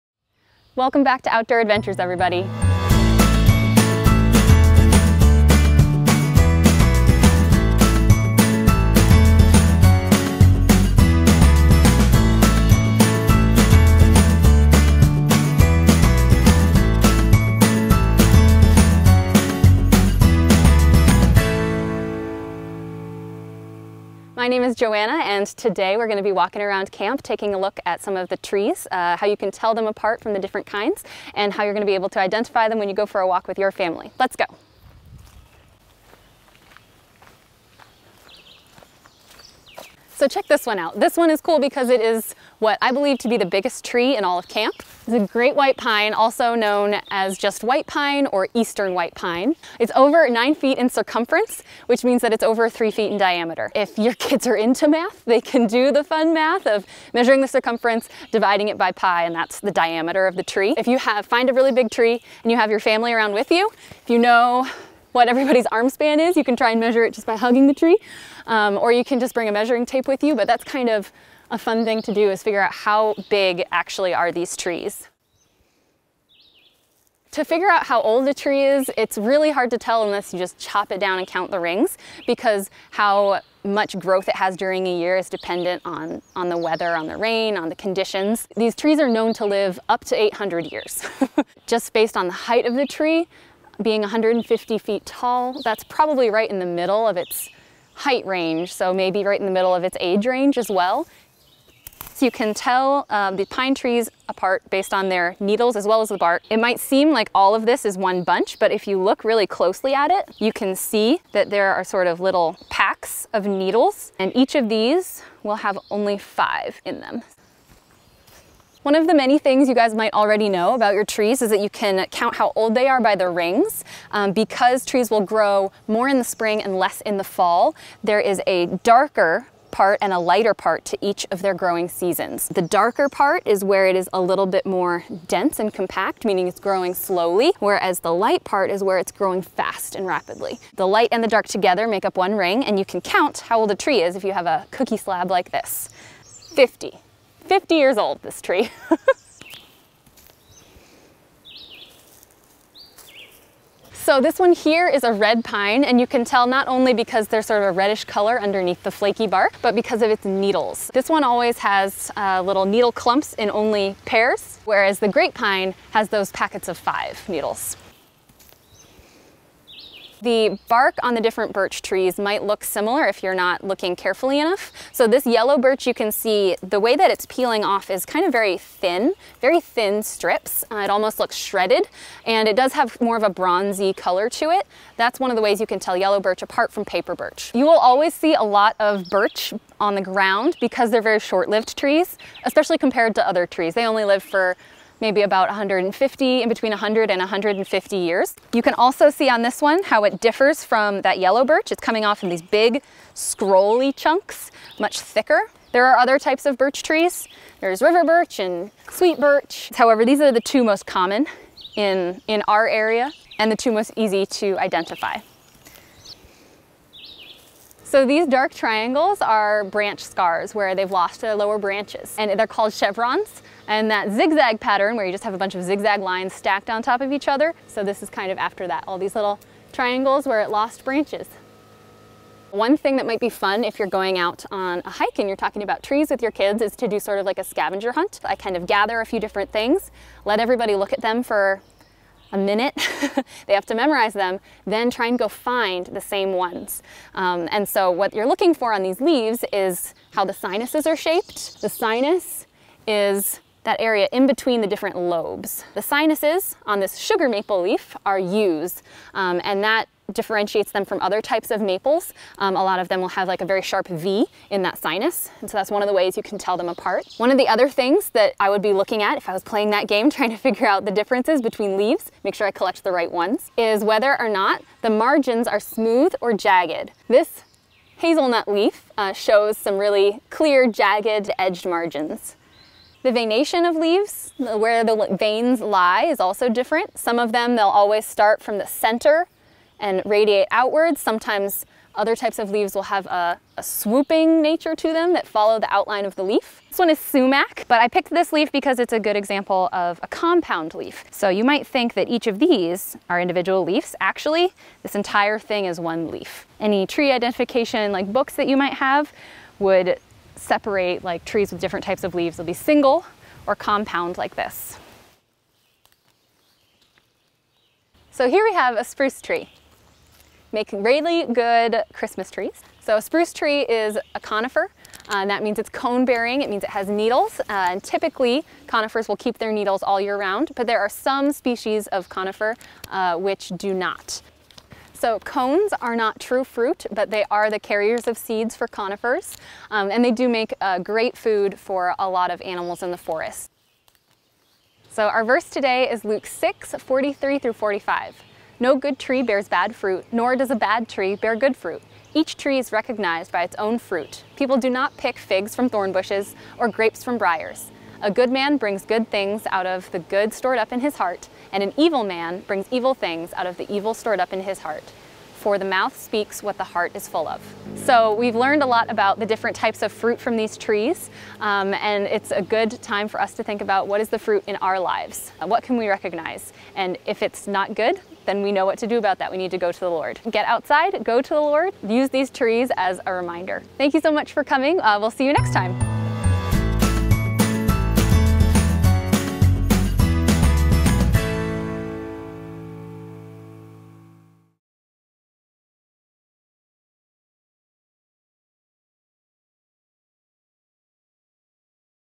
Episode 3: Trees Around Us - Forest Springs Outdoor Adventures
Trees are all around us, but how well do you know them? Come along as Joanna teaches you all about the trees of Forest Springs on this week's Outdoor Adventure!

For more info on Forest Springs, and how you can experience an Outdoor Adventure at camp, please visit us